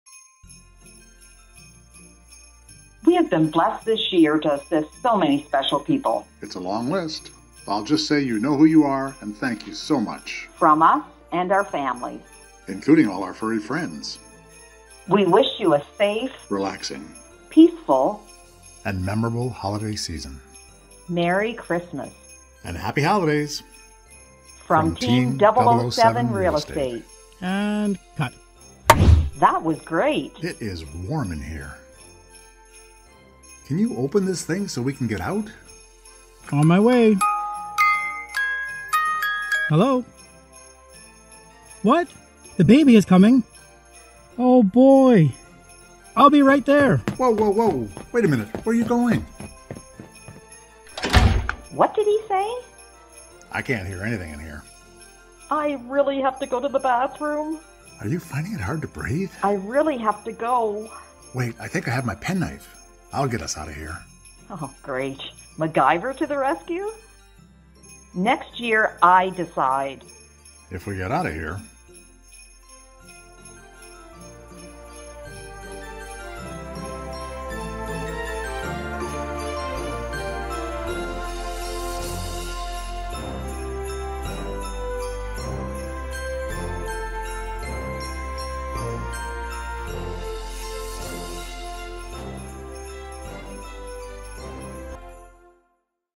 Holiday Greetings 2023
Team 007RealEstate wishes everyone a happy & prosperous holiday season!  We thank all of our past and future clients for the opportunity to assist you in buying or selling your home or property!